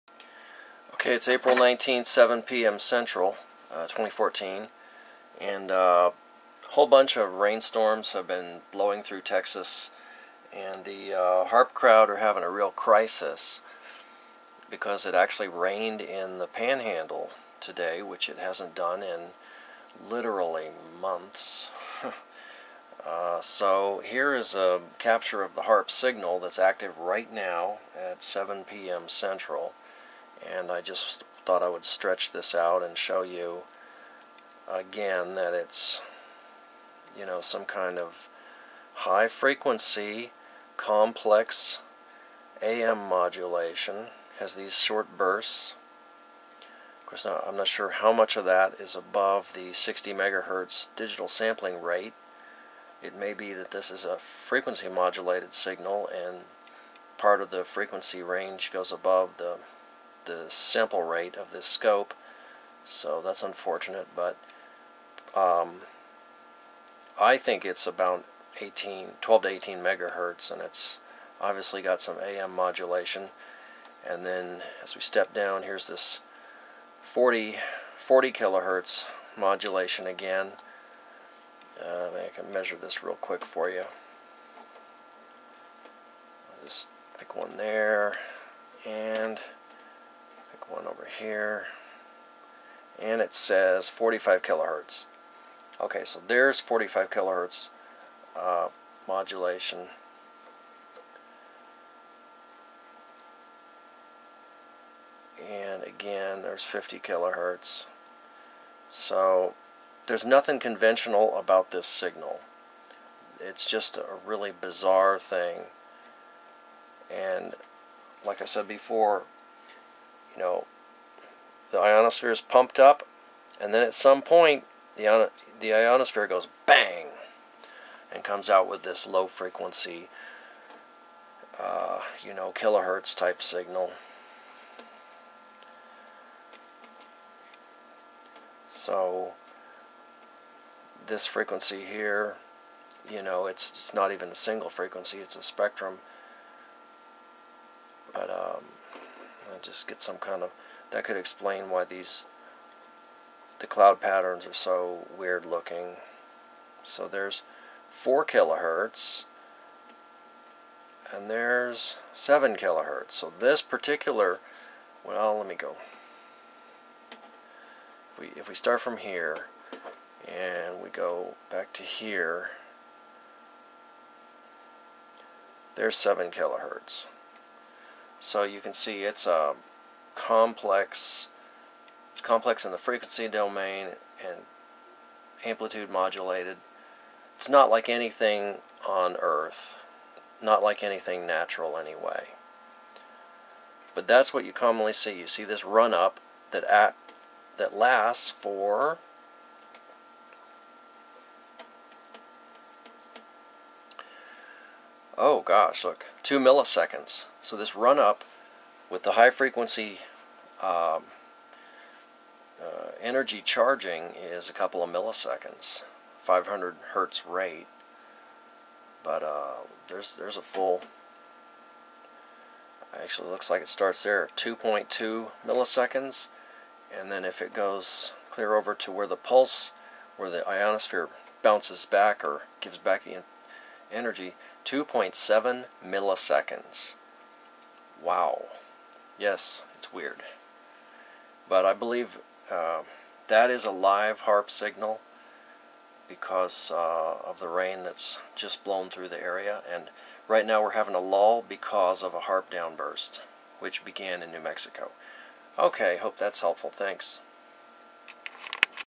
HAARP Signal on Oscilloscope part 3
More frequency measurements of the strange, AM modulated, HAARP signal. Also, the ELF radio wave from the ionosphere is looked at, showing a 7 KHz, and lower frequency spread. This group of signals takes 2 milliseconds to "pump up" the ionosphere, before the ionosphere "bounces back", which produces the ELF cloud pattern. These signals have very high power and any coil with thousands of turns can easily see it, if you put iron, or ferrite in the middle of the coil.